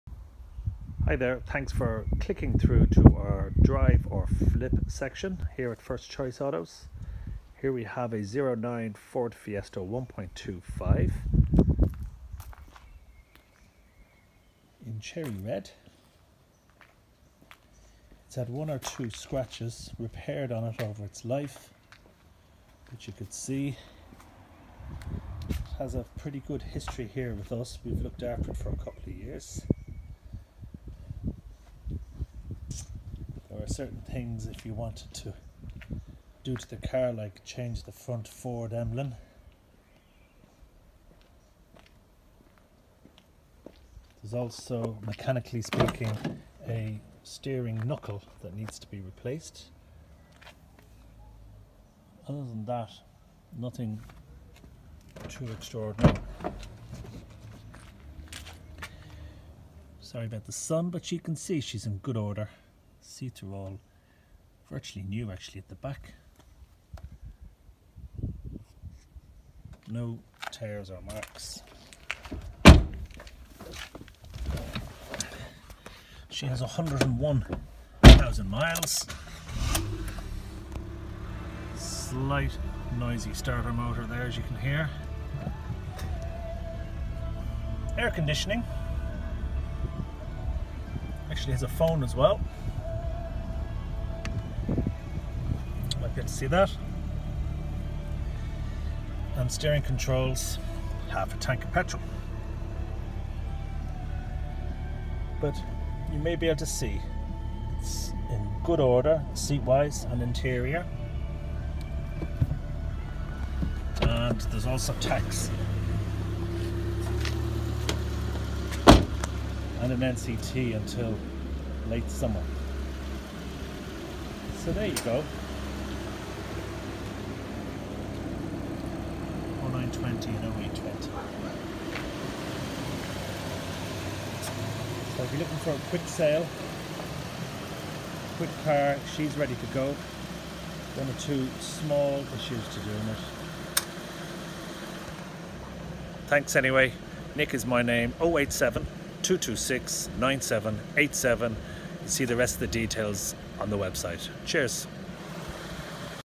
2009 Ford Fiesta 1.25
Drive or flip?!
Nick shows car – available to buy and drive at a trade price – or “budding traders “ to finish out small jobs ..